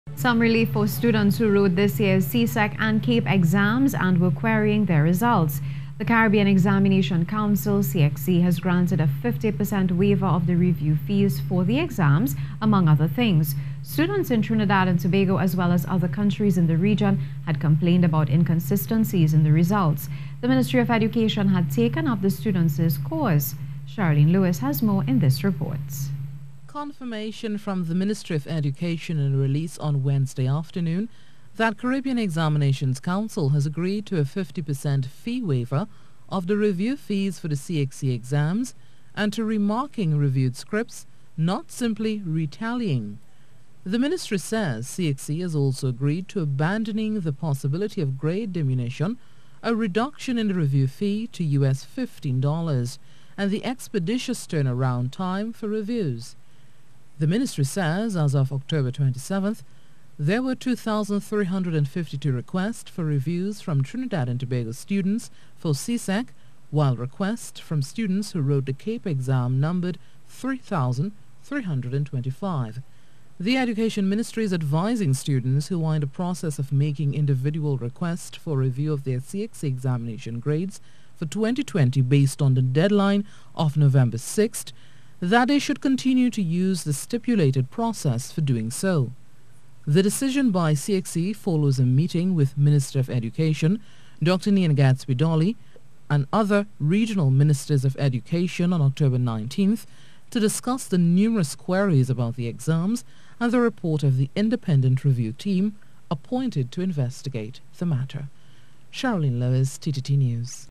CXC Grants Review Requests
Some relief for students who wrote this year's CSEC and CAPE exams and were querying their results.

The Caribbean Examination Council (CXC) has granted a 50% waiver of the review fees for the exams, among other things.

Students in Trinidad and Tobago as well as other countries in the region had complained about inconsistencies in the results.

The Ministry of Education had taken up the students' cause.

Cherrylene Lewis has more in this report.